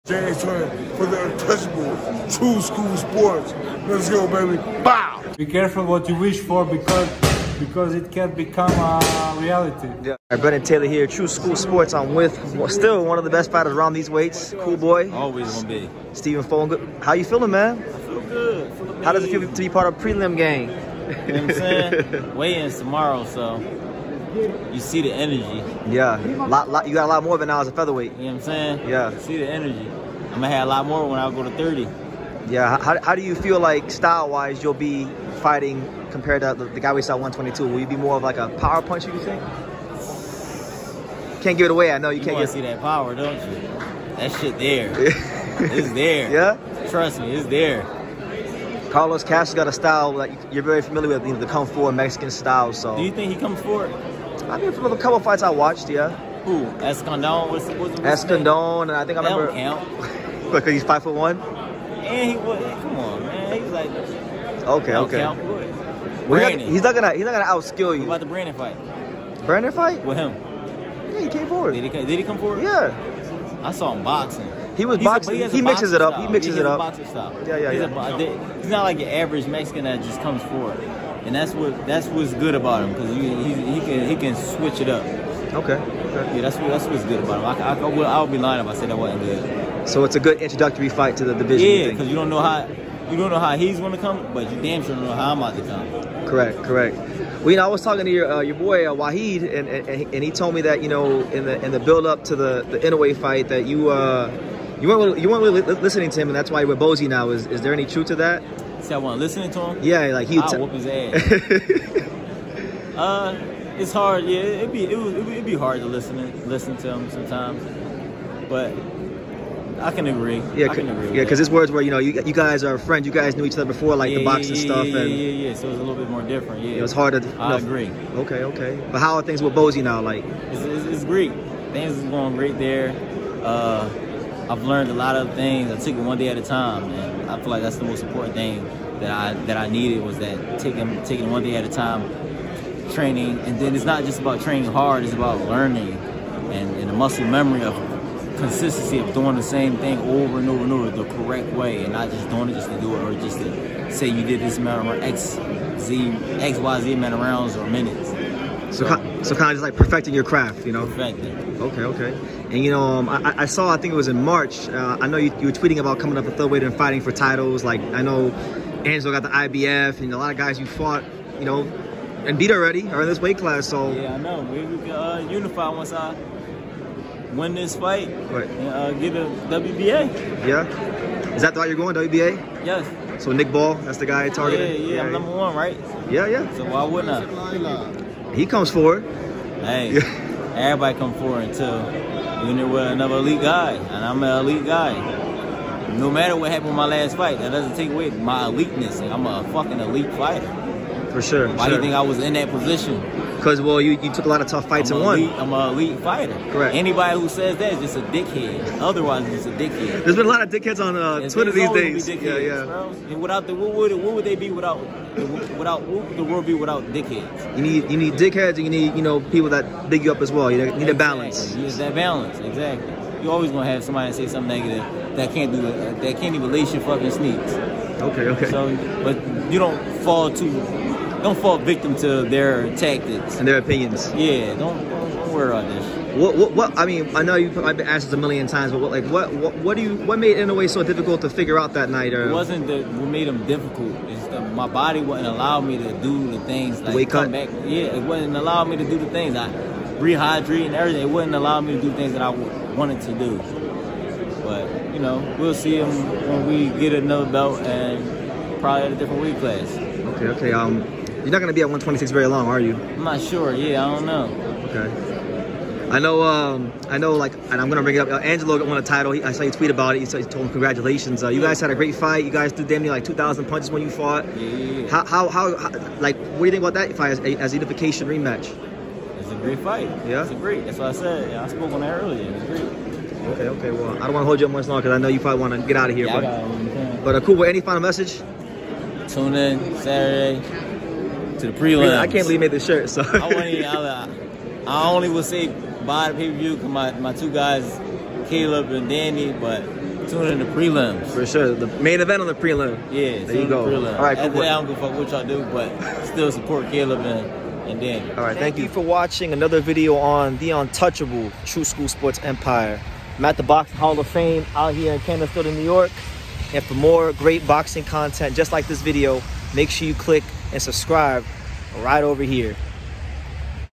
STEPHEN FULTON REVEALS WHAT WENT WRONG IN NAOYA INOUE 井上 尚弥 FIGHT & TARGETING NICK BALL!!!
Had a chance to catch up with Stephen Fulton ahead of his featherweight debut and first fight back to since his loss to Naoya Inoue. Good taking to Coolboy as always!